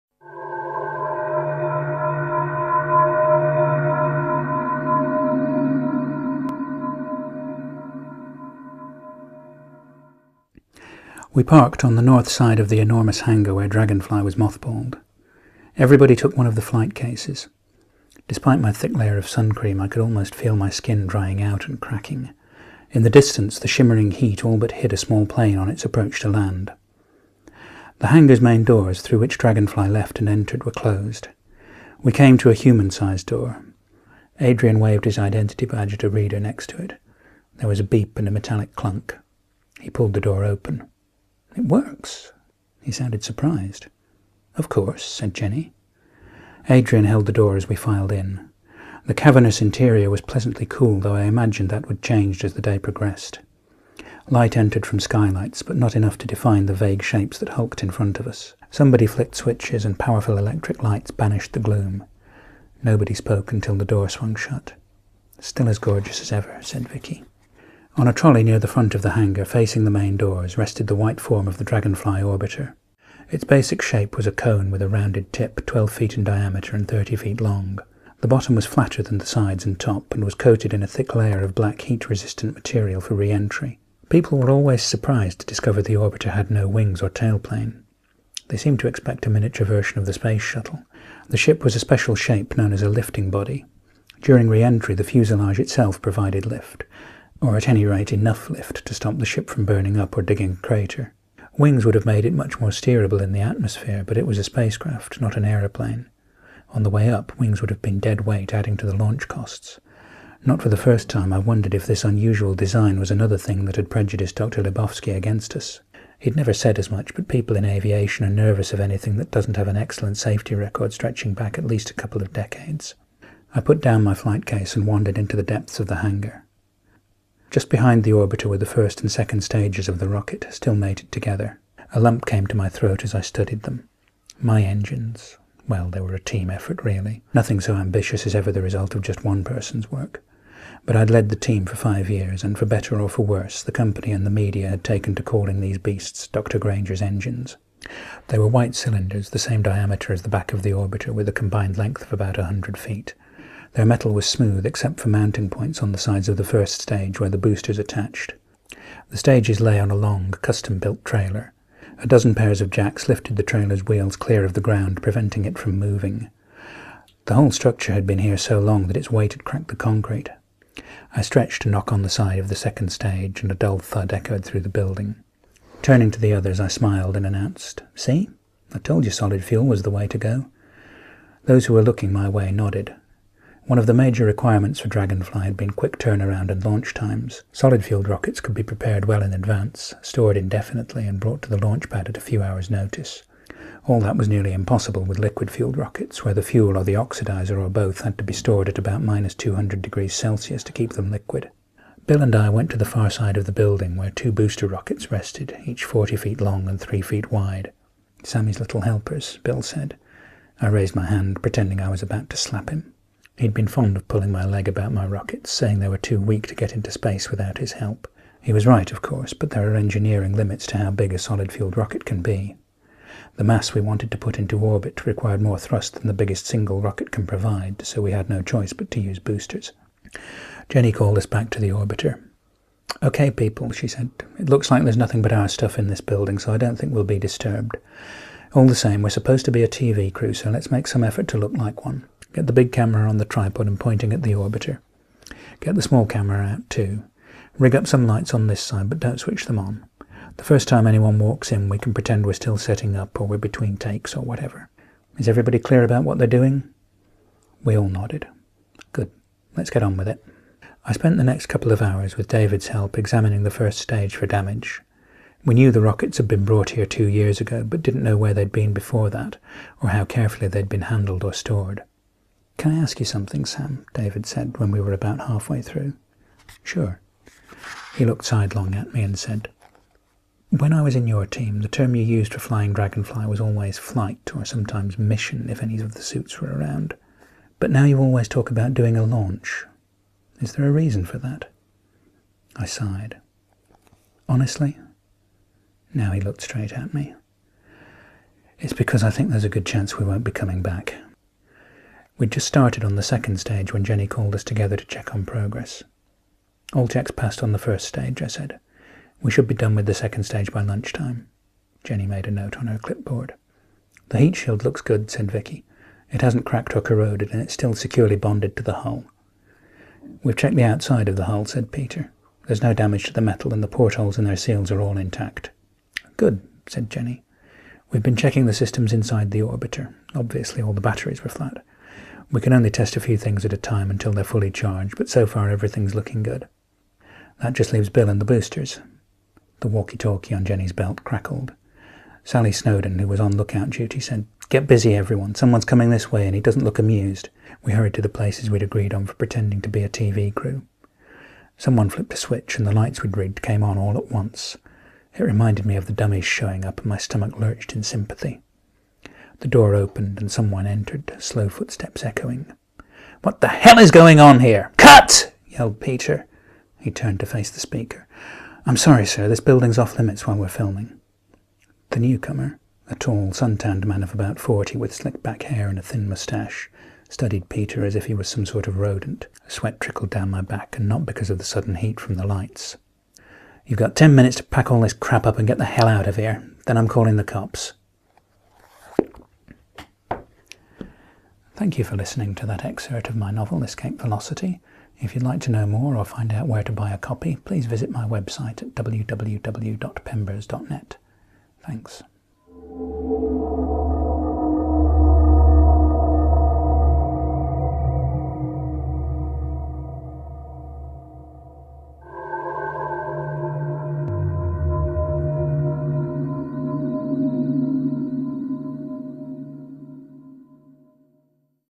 Casing the Joint - a reading from Escape Velocity by Steven J Pemberton
The author reads an extract of his science fiction novel Escape Velocity. for a free sample and to find out where to buy a copy.

Sam and Jennie Grainger have learned that the key to mending the holes in the world may be the spaceship they worked on before Jennie died. Which wouldn't be so bad if they didn't have to steal it...